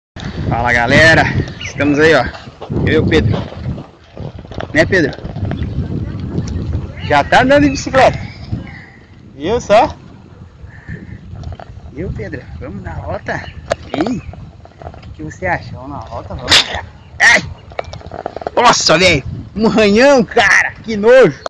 Riding Dirty || ViralHog
Occurred: August 3, 2016 / Joinville, Brazil

Info: A dad takes his child out for a bike ride, and the baby shoots snot out of both nostrils.